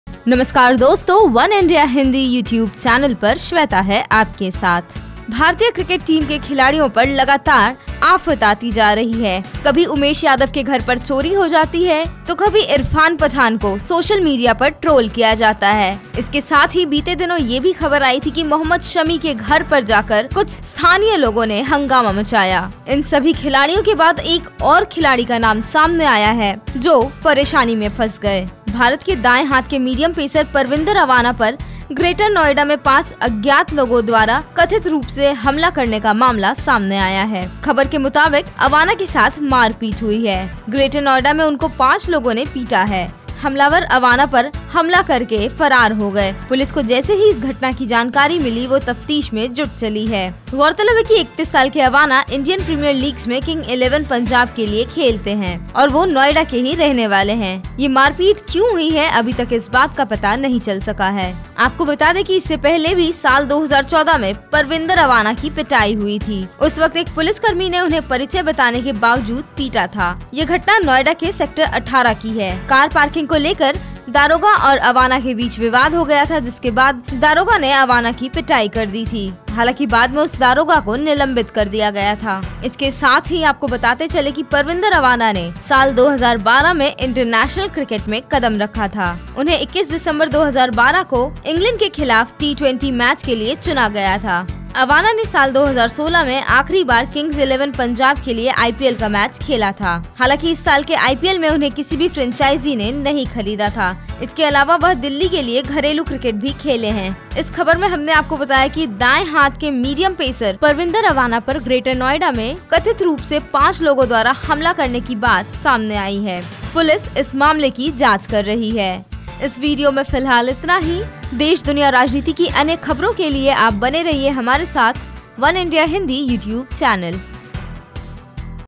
Indian Cricketer Parvinder Awana Attack by Unknowns in Greater Noida । वनइंडिया हिंदी
Indian right hand medium Pacer Parvinder Awana was beaten up by some unknown people's in Greater Noida. Police is investigating the entire story.

भारतीय क्रिकेट खिलाड़ी परविंदर अवाना पर ग्रेटर नोएडा में अज्ञात लोगों द्वारा हमला किए जाने का मामला सामने आया है. इस हमले की ख़बर मिलते ही पुलिस जांच में जुट चली है. इसके पहले भी अवाना की पिटाई का मामला सामने आया था.